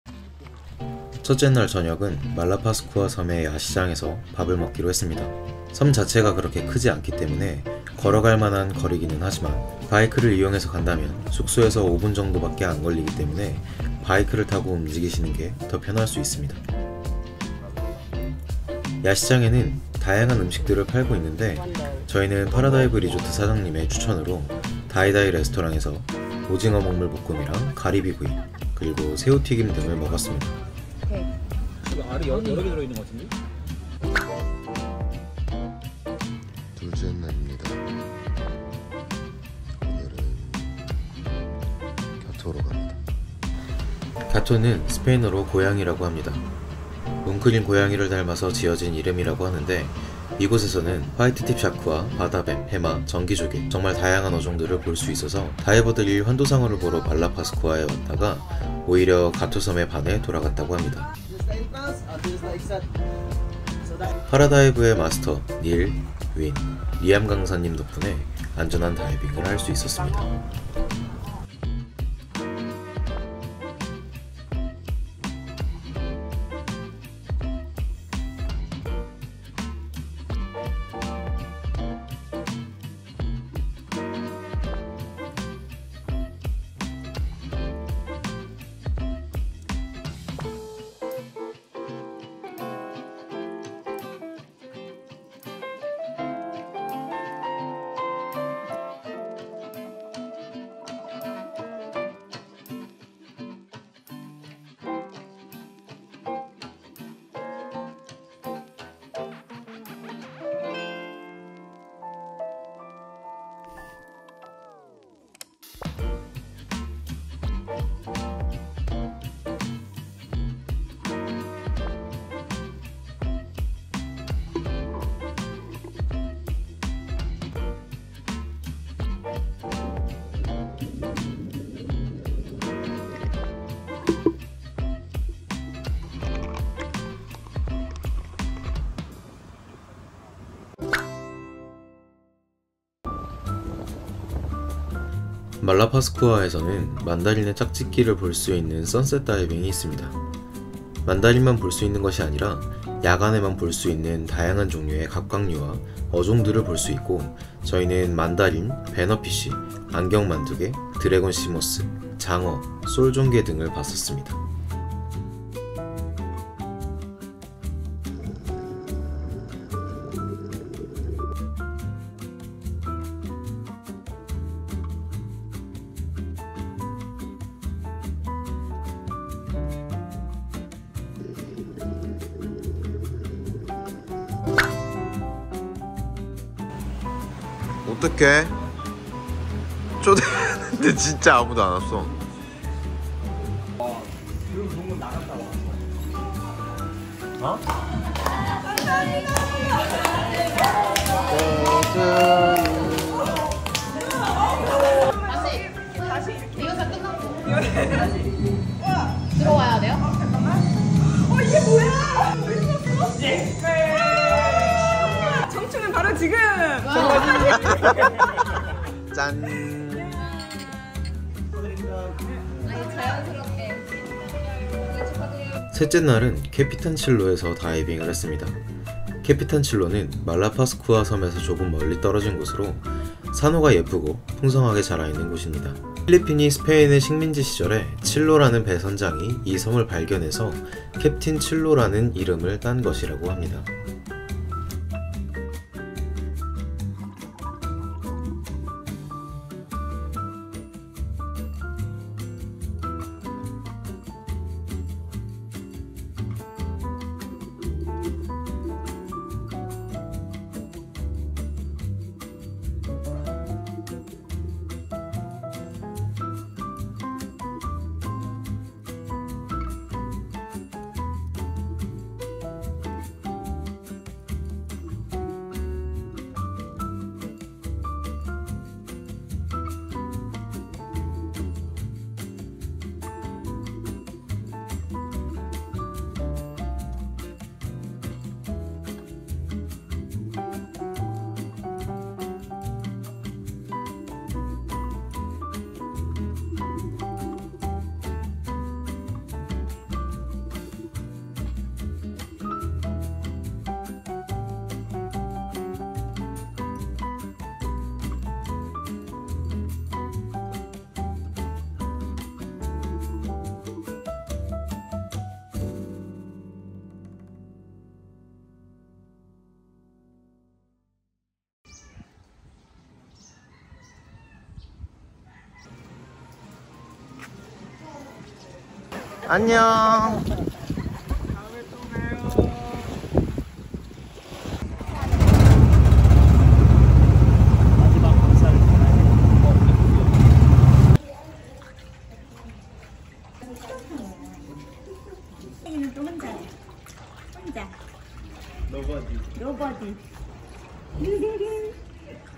아는사람만 찾아간다는 이 섬! 사실 말라파스쿠아 섬은 '환도상어'보다 더 매력적인 곳들이 많다는것! ㅣ필리핀 말라파스쿠아 섬
안녕하세요. '퐁당'입니다.
말라파스쿠아에는 환도상어를 볼 수 있는 키무드숄, 모나드숄 뿐만 아니라 만다린을 볼 수 있는 선셋다이빙, 화이트팁 샤크와 다양한 어종들을 볼 수 있는 갸토와 산호가 풍성하게 자라있는 케피탄칠로까지 정말 많은 포인트 들이 숨어있습니다.
기회가 된다면 일주일 정도 머무르면서 못가본 포인트들까지 둘러보고싶을정도였어요. 아쉽기는 하지만 다음번에 또 오면 되니까요!

✔️TimeLine
00:00 야시장
00:36 가토 Point
02:23 만다린 선셋 다이빙
03:06 생일파티
03:46 케피탄칠로 Point
05:22 뽕댕이 No buddy

✔️트루다이브 슈트 구매는